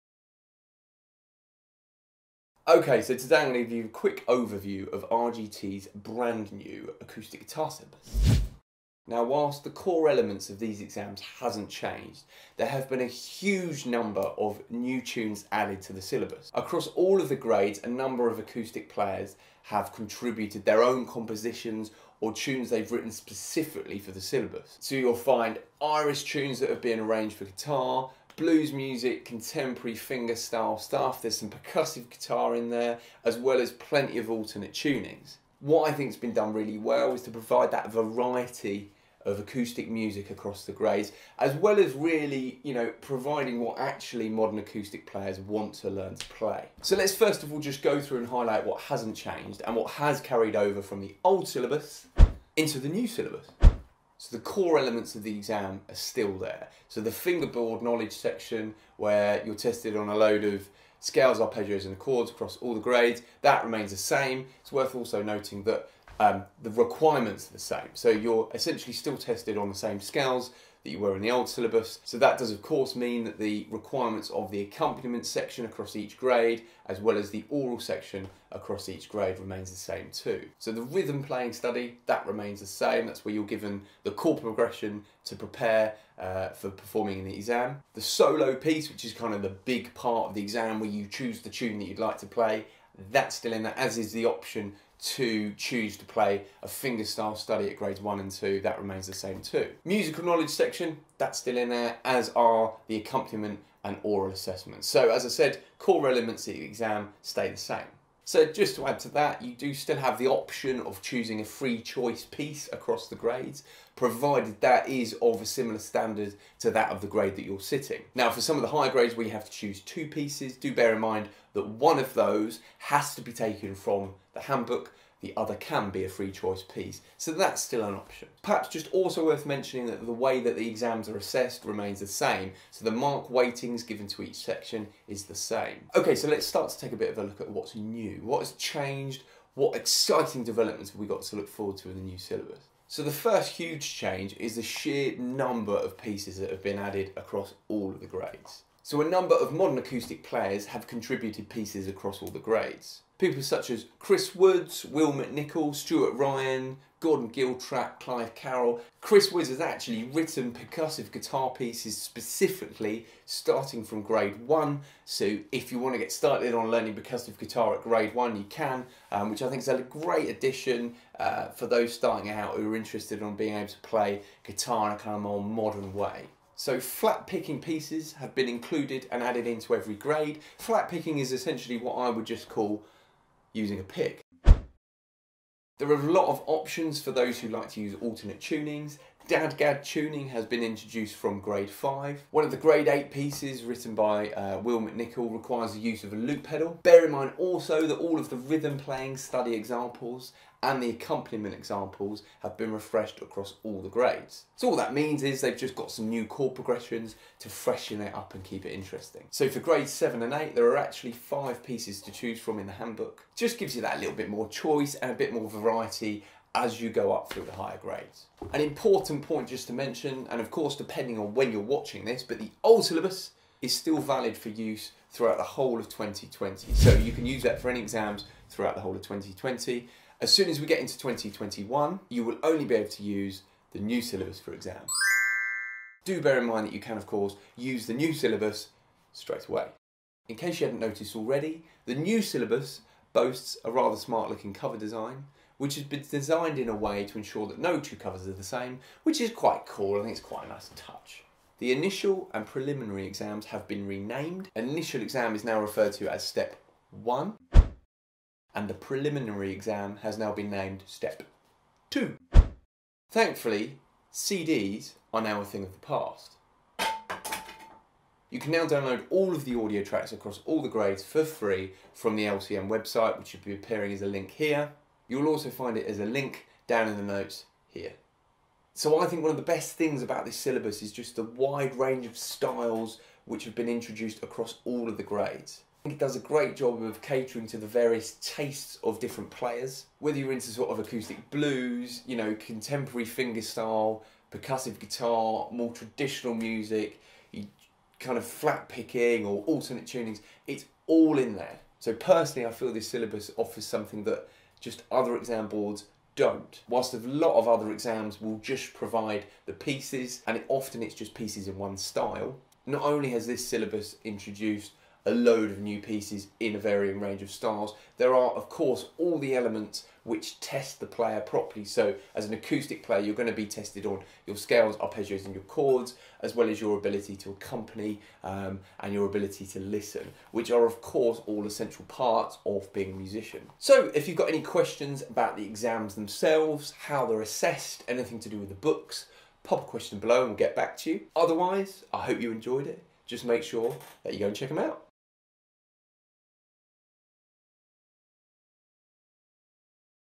RGT Acoustic Guitar Syllabus Overview
LCME Examiner and Guitarist, Charlie Tottman provides a quick overview of our new, and super cool, Acoustic Guitar Syllabus!